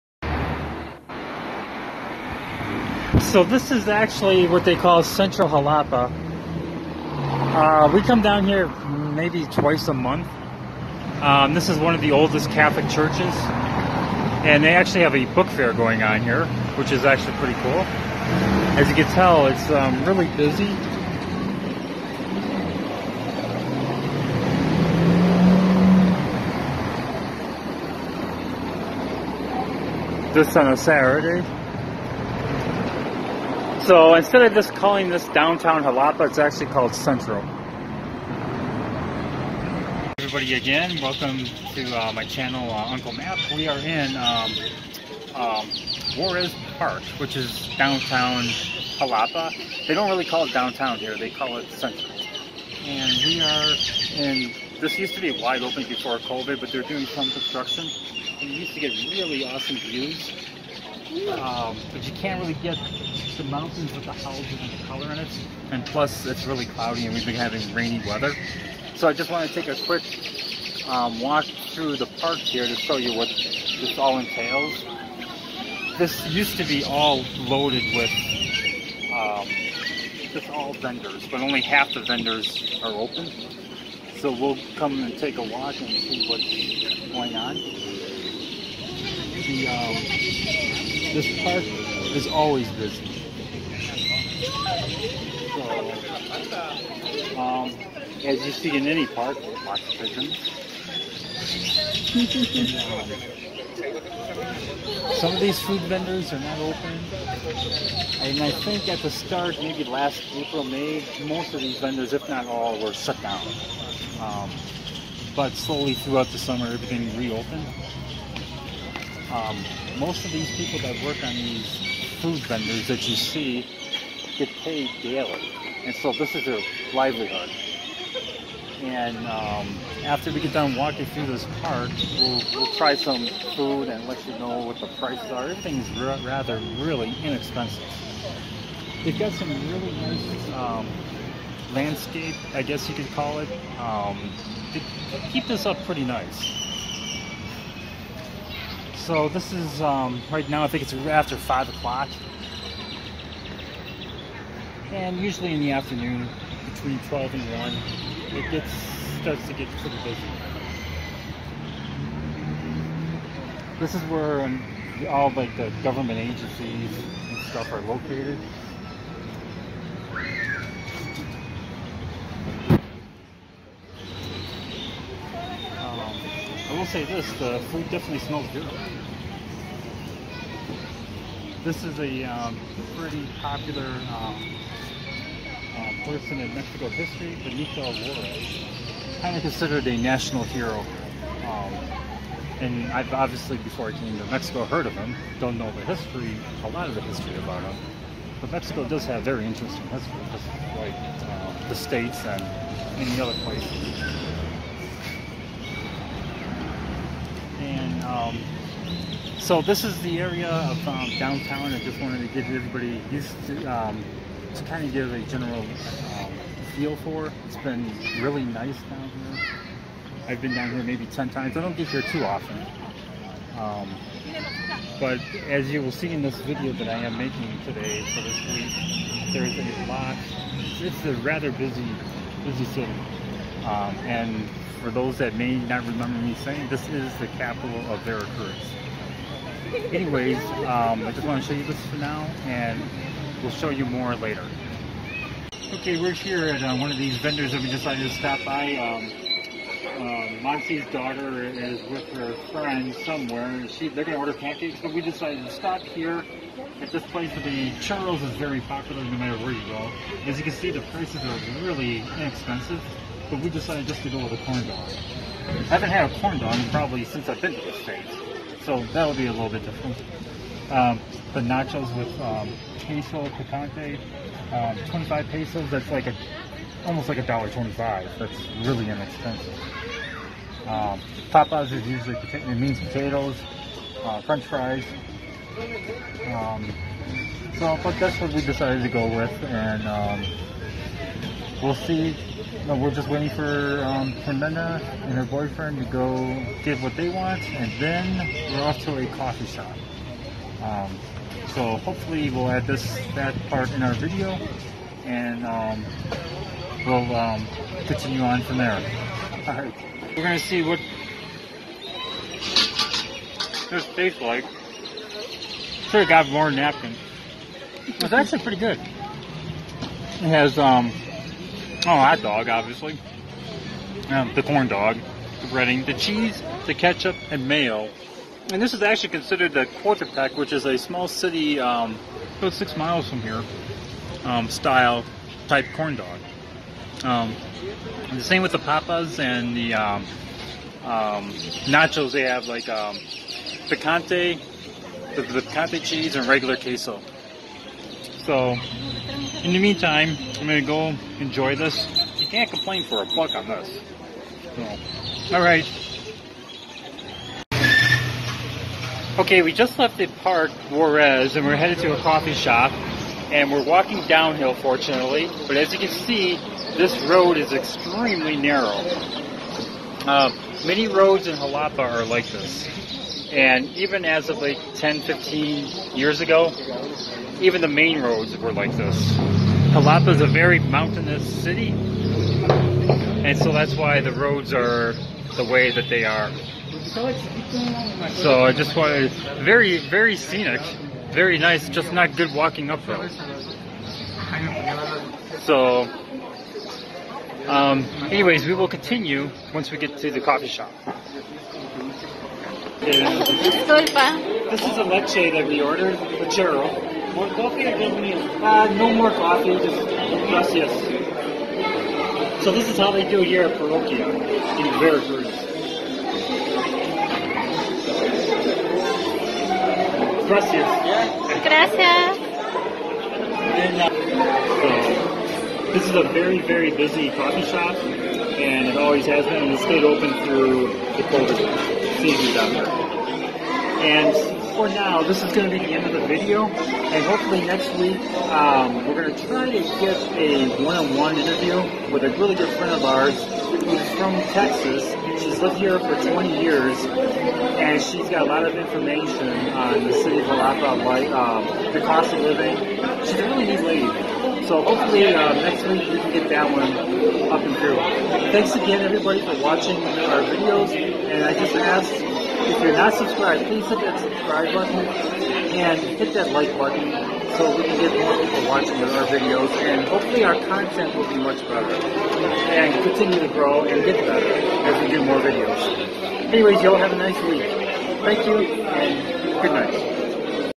A Coffee Afternoon in Downtown Xalapa.
The park "Enriquez" in Downtown Xalapa, where Xalapeños gather to enjoy an afternoon and one of the most iconic coffee shops in Veracruz with of course a delicious "lechero" .